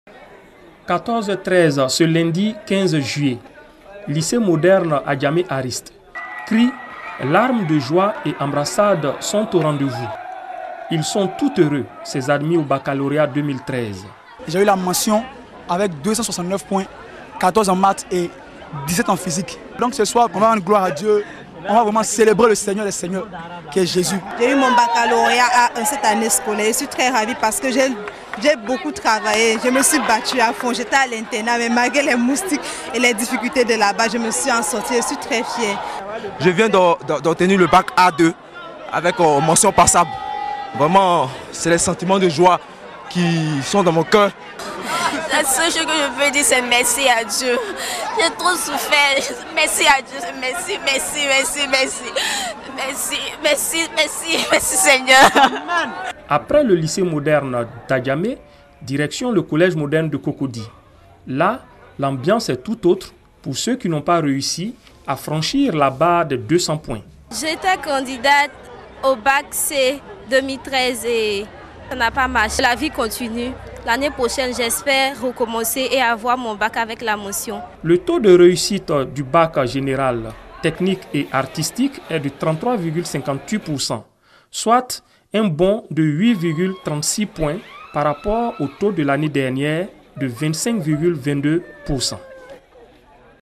Baccalauréat 2013: Ambiance des écoles après la proclamation des résultats
Education/ Baccalauréat 2013: Ambiance des écoles après la proclamation des résultats